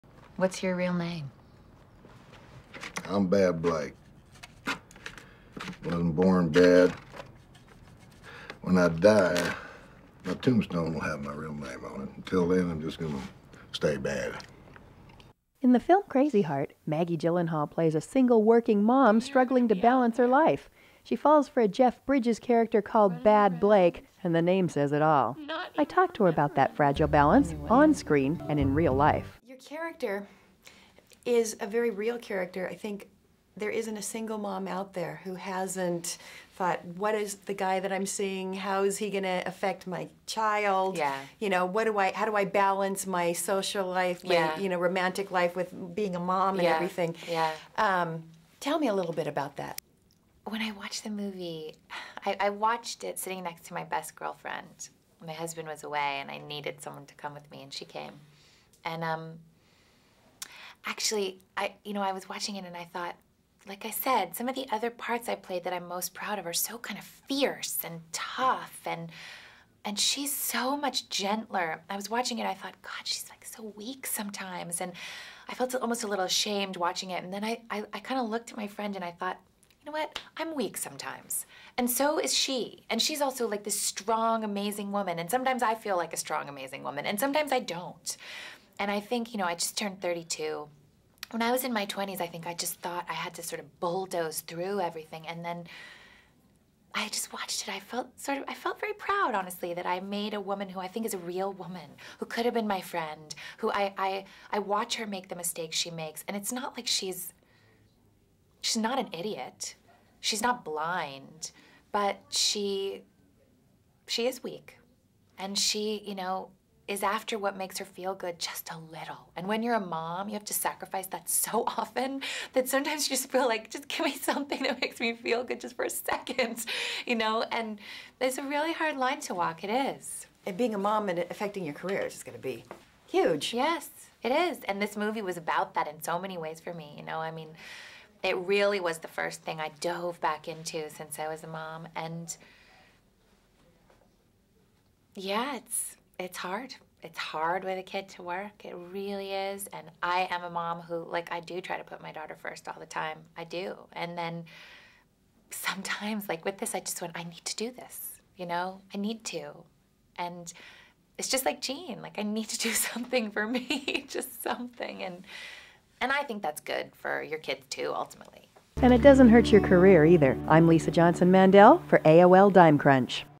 Maggie Gyllenhaal talks about Crazy Heart and crazy life
In the film Crazy Heart, Maggie Gyllenhaal plays a single, working mom who has to make some tough decisions. She talks about how balancing work and family is challenging in real life as well.